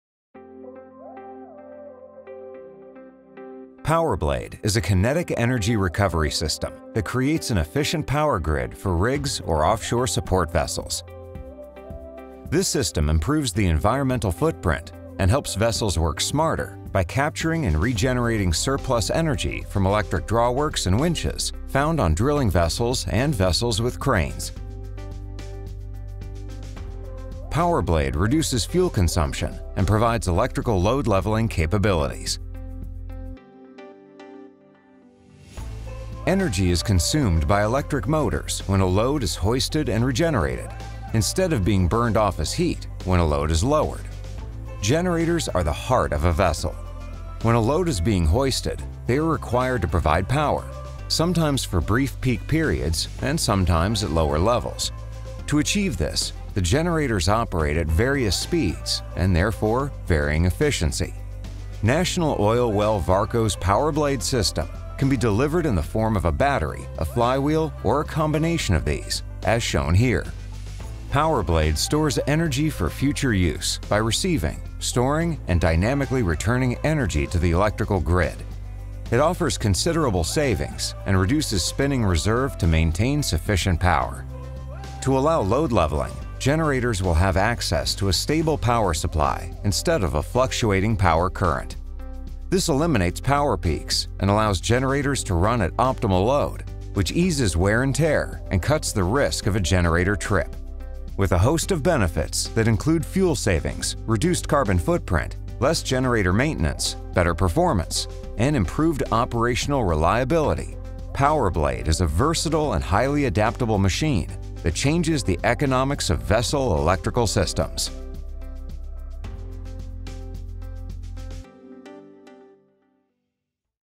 Powerblade Kinetic Energy Recovery System on Installation Vessels
NOV's Powerblade being used with the cranes and towers on windfarm development vessels to capture and regenerate energy during operations.